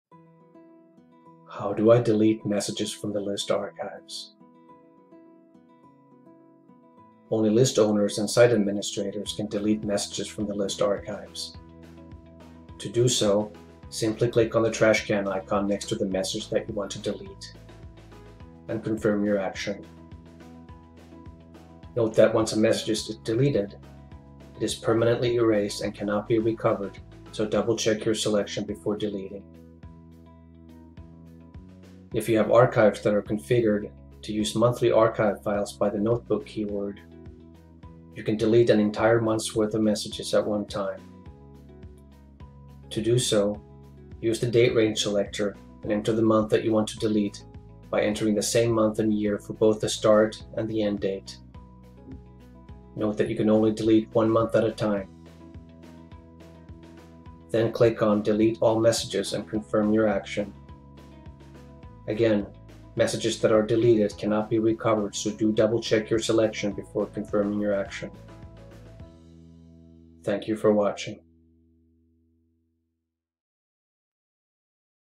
How to delete messages from the list archives in LISTSERV®
This guide shows how email list owners or administrators can delete messages from the list archives, either one-by-one or an entire month at a time.

Knowing how to keep your list archives clean and organized and where functions are located on the LISTSERV web interface saves you time and makes your list management tasks more enjoyable.